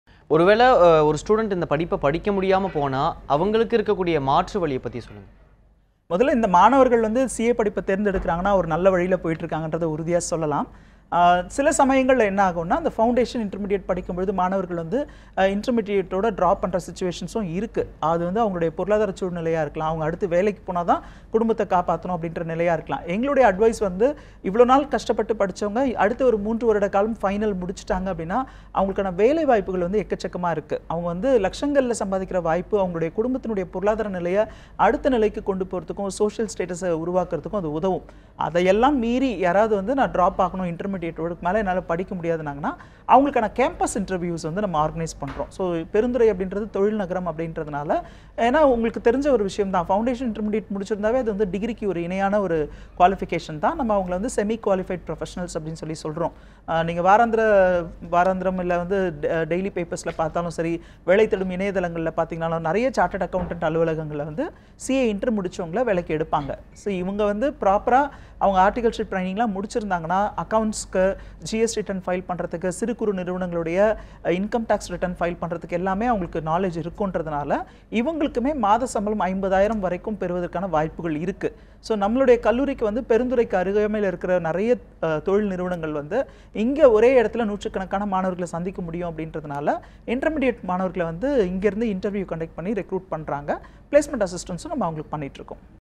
CA,CMA படிப்புகளை பாதியில் நிறுத்தியவர்களுக்கு மாற்று வழி என்ன?
CA,CMA படிப்புகளை பாதியில் நிறுத்தியவர்களுக்கு மாற்று வழி என்ன? - NEWS7 தமிழ் தொலைக்காட்சி நேரலையில் விரிவான விளக்கம்  அளிக்கிறார் நேஷனல் மேனேஜ்மென்ட் கல்லூரியின் மாணவர் சேர்க்கை ஆலோசகர் - திரு.G.மணிகண்டன்

👉 2023-24 Admissions Open for CA, CMA Courses!
🏢National Management College,
Thudupathi, Perundurai, Erode.